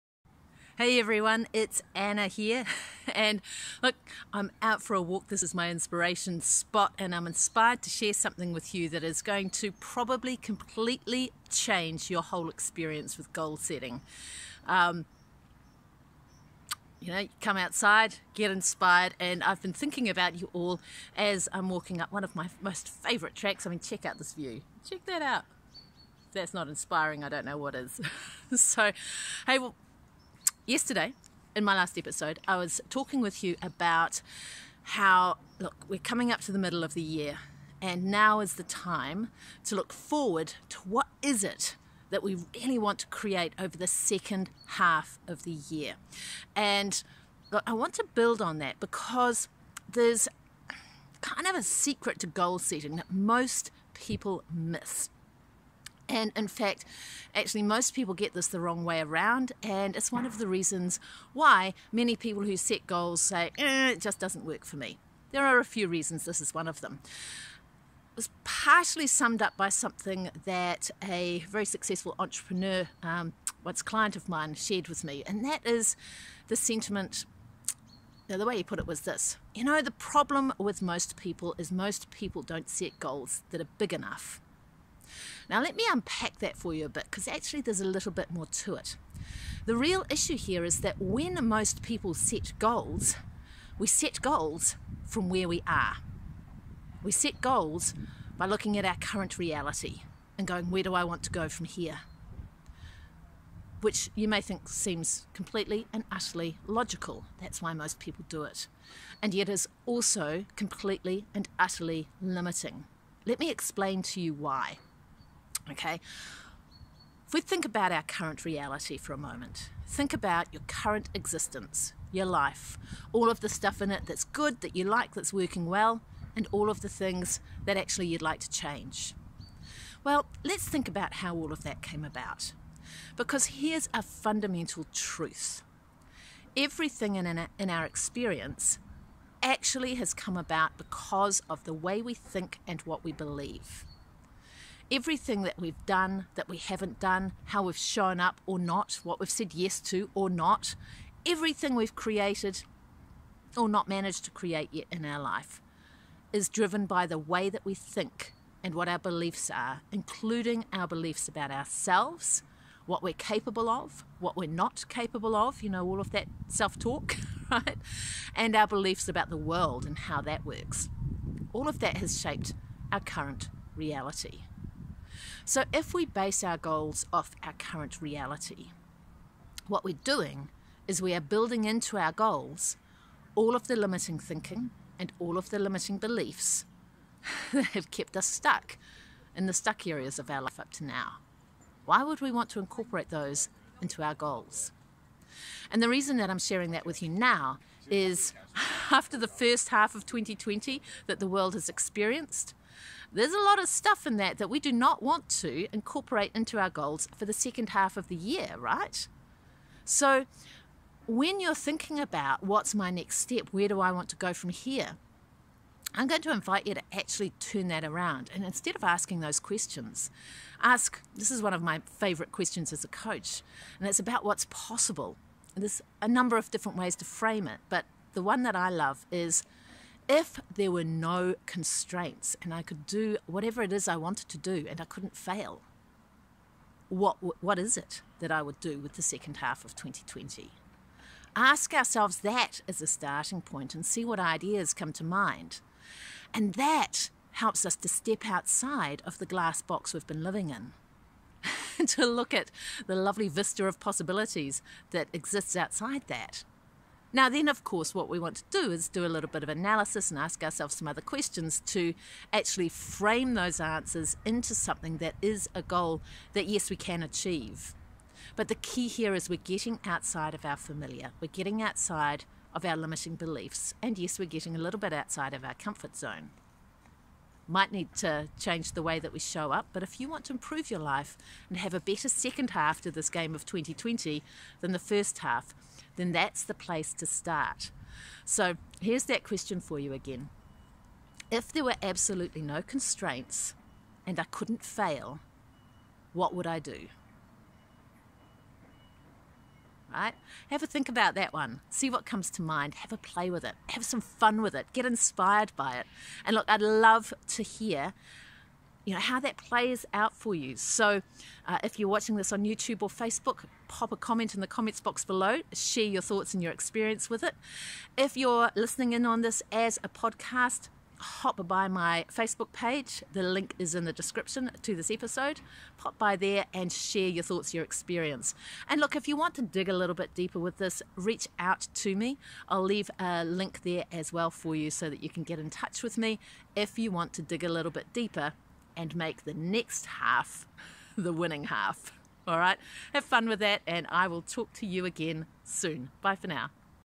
Most Common Goal Setting Mistake
There is one common goal-setting mistake, that most people make. And that really holds you back from what you are truly capable of.
Listen to find out what it is, and how to avoid it.
And share your insights in the comments below.